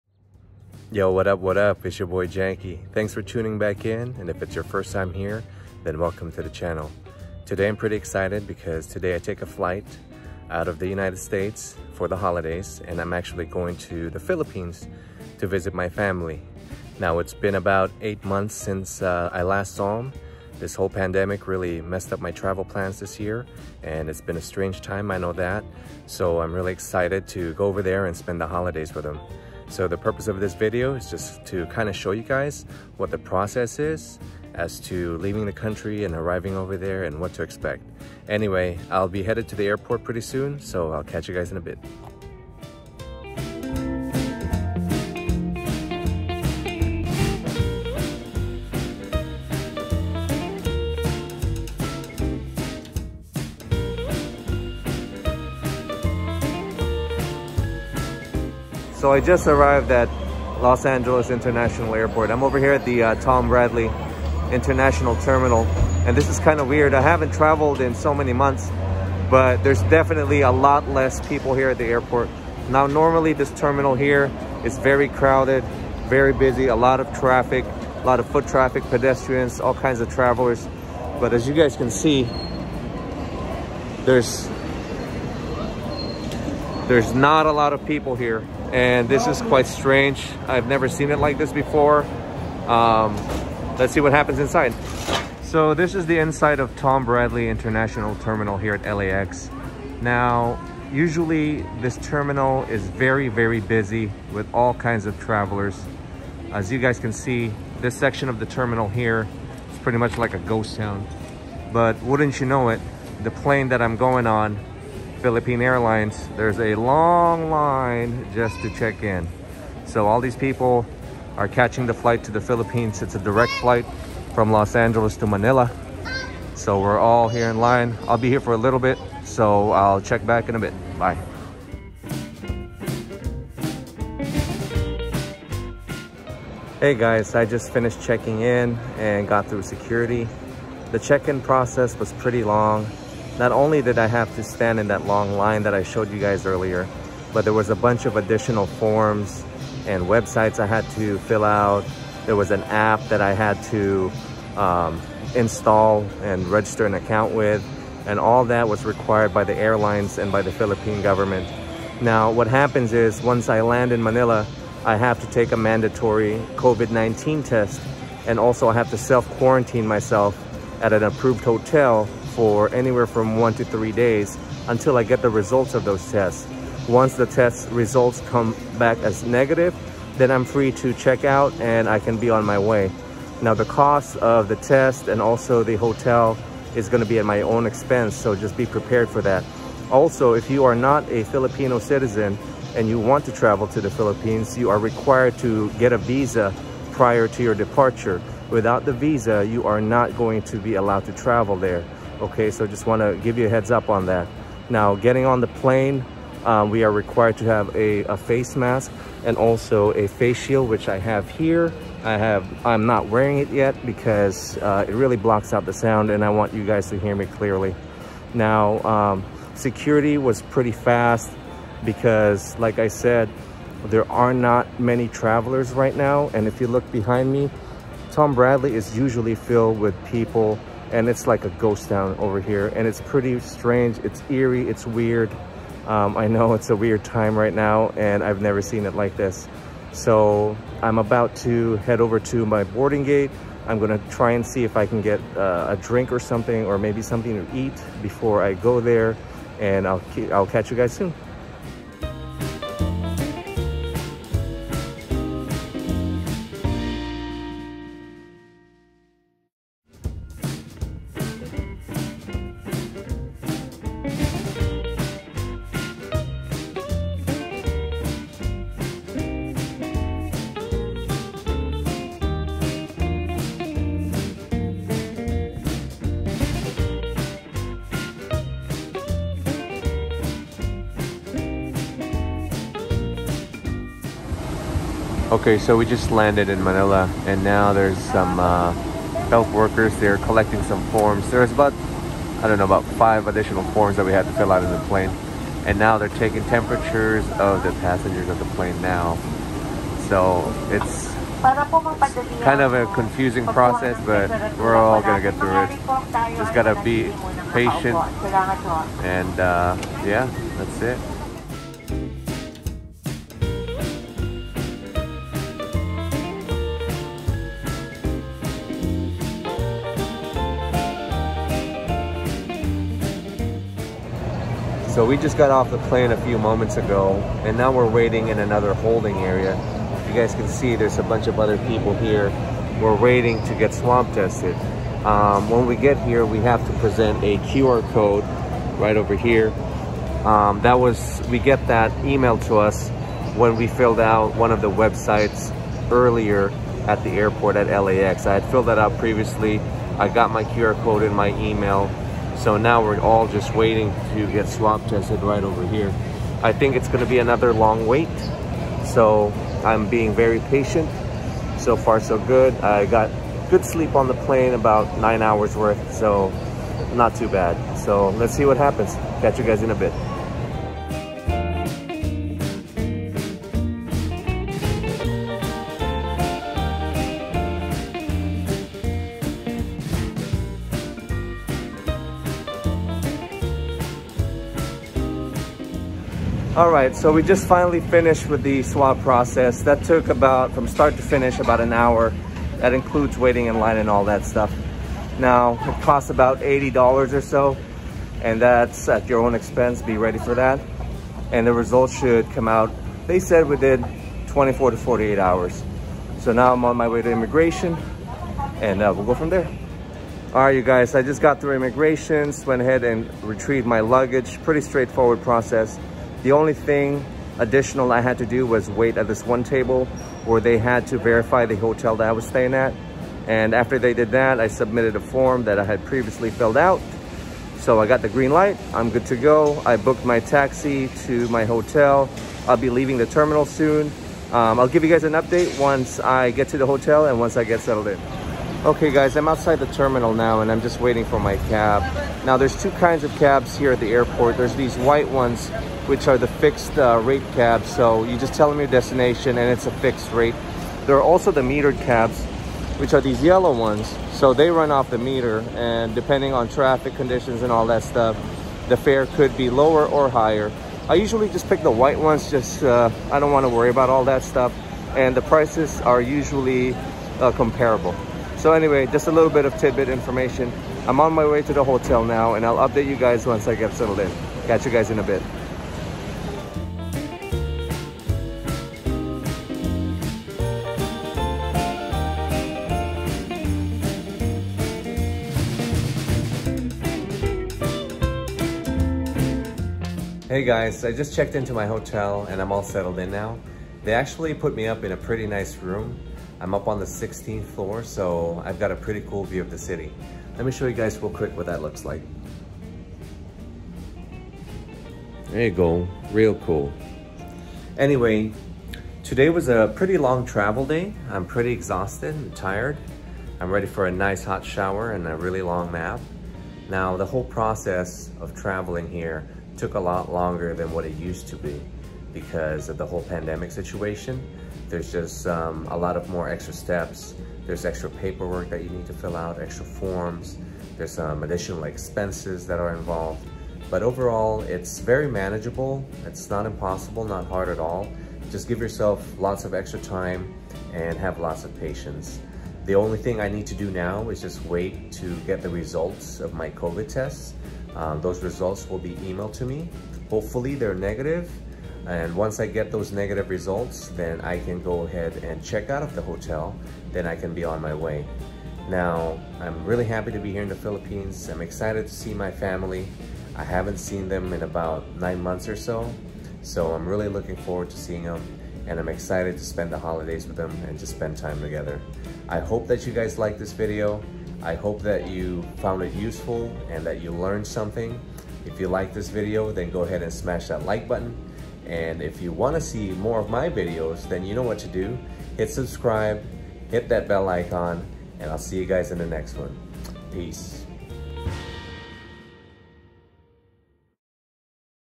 Traveling to the Philippines During the Pandemic!
I wanted to share with you guys my travel experience to the Philippines during the pandemic, so you can know what to expect.

and click on the bell icon so you can get notified whenever I make new videos.